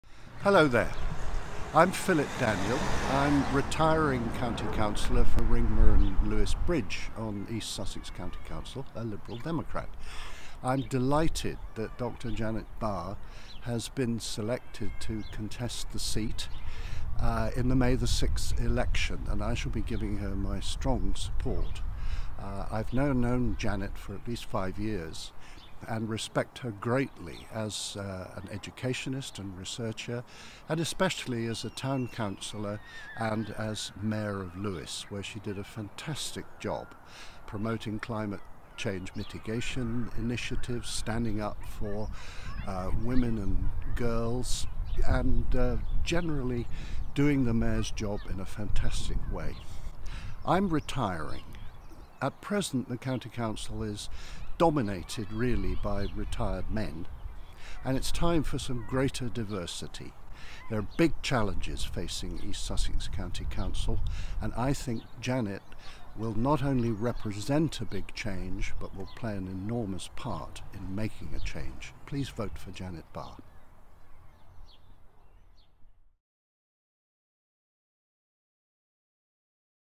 Philip Daniel: It's time for some greater diversity
Retiring East Sussex County Councillor Philip Daniel, a Liberal Democrat, says that it's time for greater diversity in ESCC.

"There are big challenges facing ESCC - and I think that Janet will not only represent a big change, but will play an enormous part in making a change."